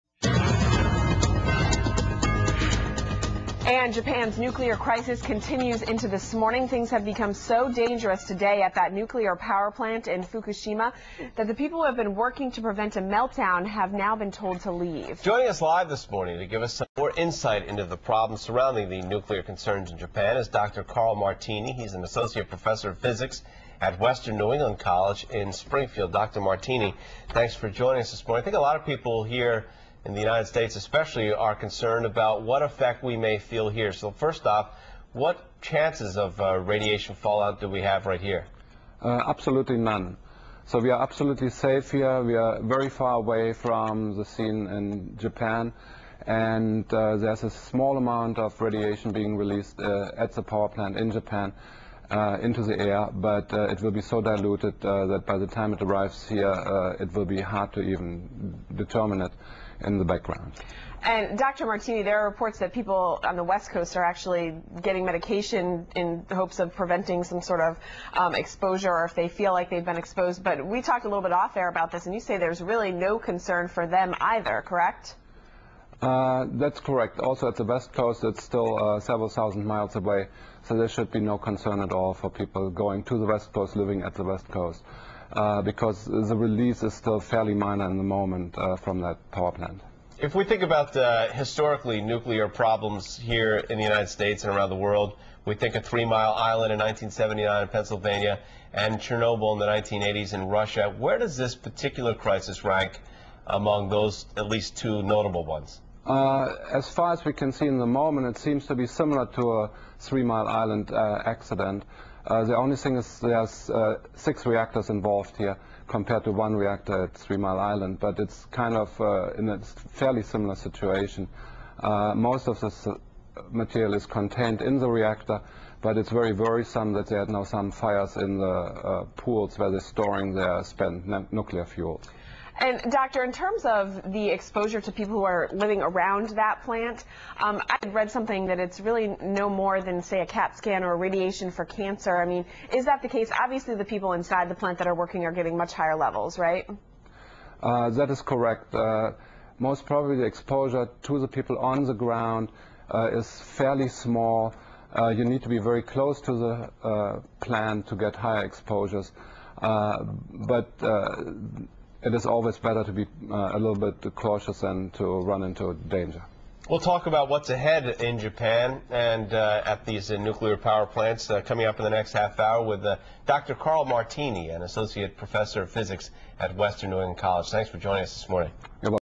Professor Karl Martini Discusses Nuclear Threats
Associate Professor of Physics Karl Martini discusses the nuclear threat in Japan as a result of the earthquake.